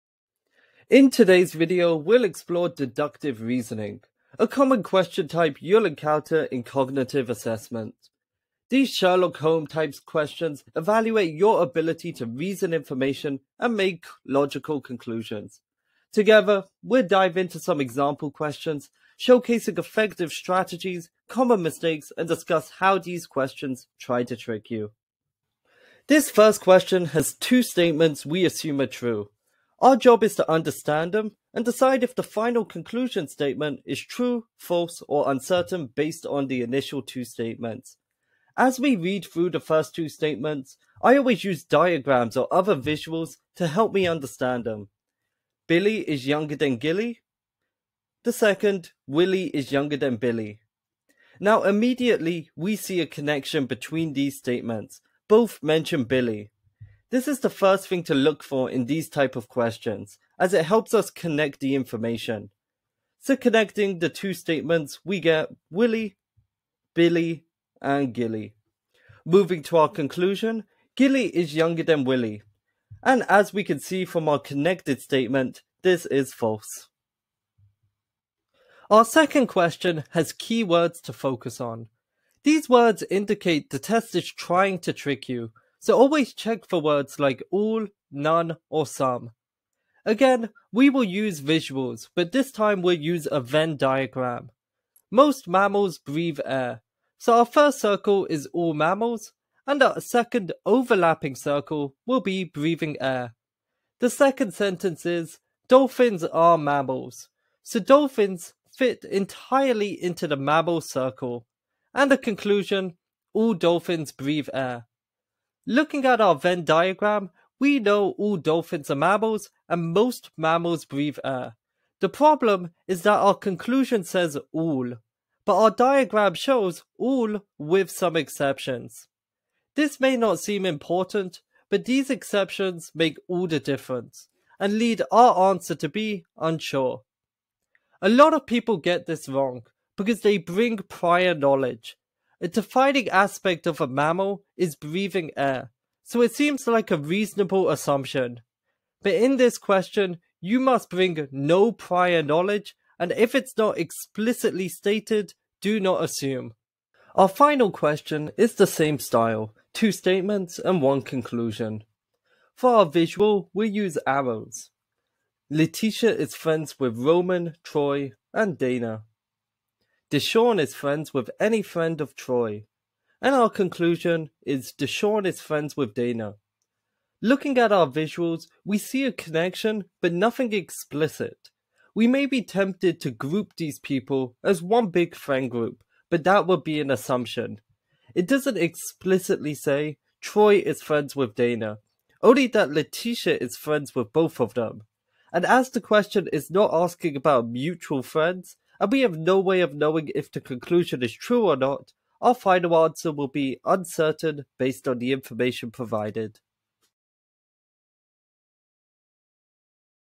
Deductive Reasoning Question Type
Understand what Deductive Reasoning questions are and get tips for succeeding on your test.

For more lessons and additional information on Cognitive Assessments, visit iPrep's preparation course